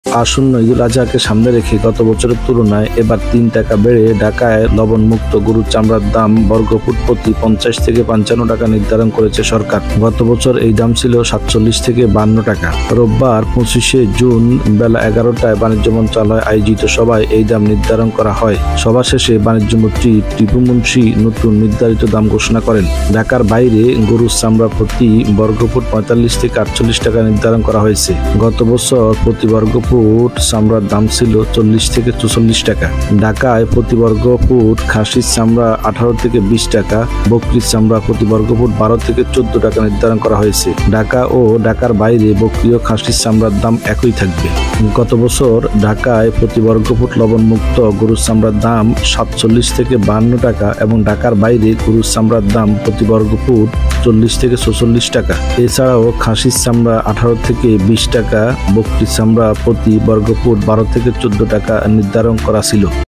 দেশে কোরবানির পশুর চামড়ার দাম নির্ধারণ। কোরবানী । চামড়ার নতুন দাম ।
আসন্ন ঈদুল আজহাকে সামনে রেখে গত বছরের তুলনায় এবার তিন টাকা বেড়ে ঢাকায় লবণযুক্ত গরুর চামড়ার দাম বর্গফুট প্রতি ৫০ থেকে ৫৫ টাকা নির্ধারণ করেছে সরকার। গত বছর এই দাম ছিল ৪৭ থেকে ৫২ টাকা।